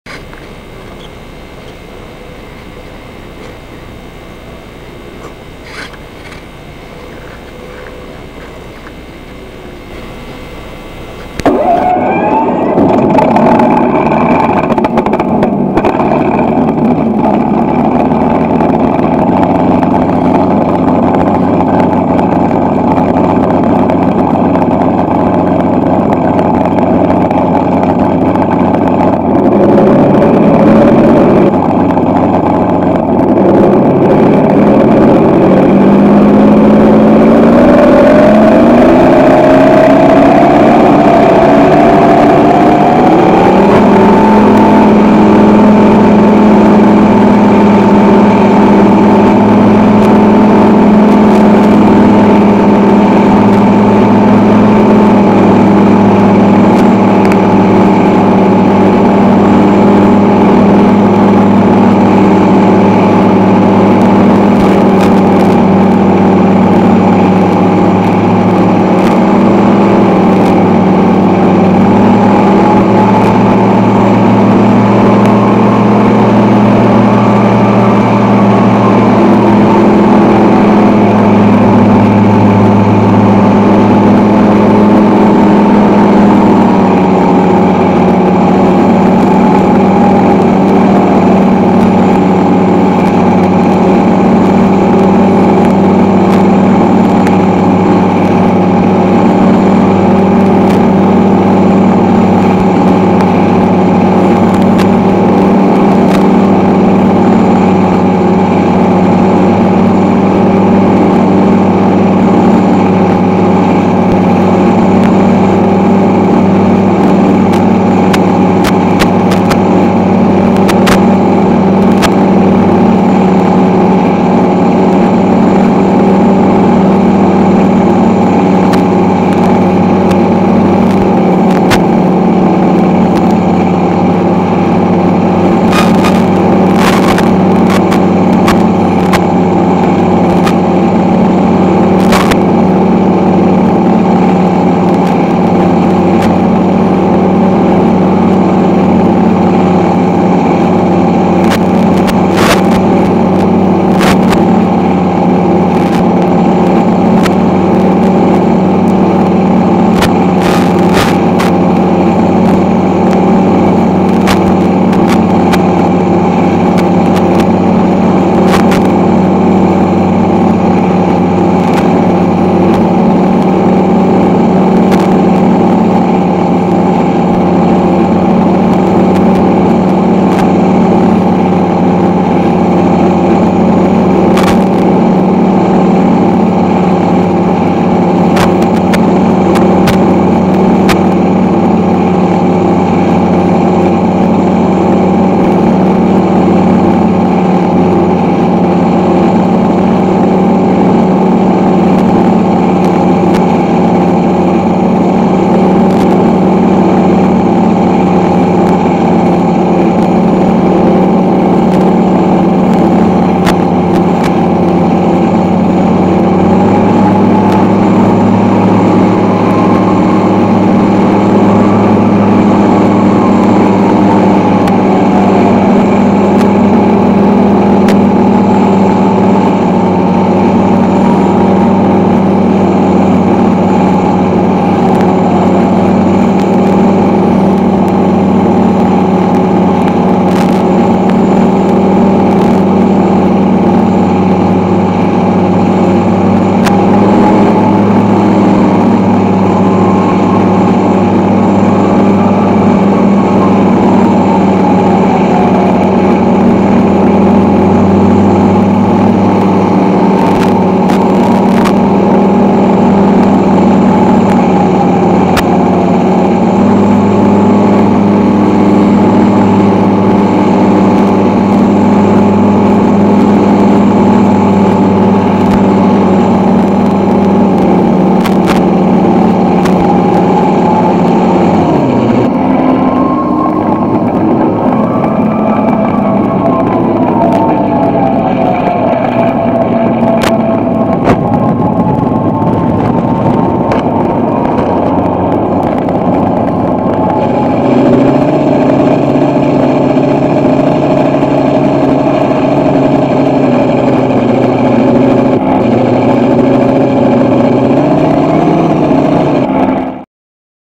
P51 (RC) at Joe Nall 13 May 23
Another fun flight of Iron Ass, this time at Triple Tree Aerodrome. Vibration is caused by a loose motor mount, again. Have to fix that video static. Oooops, hit the fly by button before landing. The day looked pretty gloom at first, but cleared up nicely.  I met a lot of nice pilots. Have fun at Nall, all week, but I had to return home.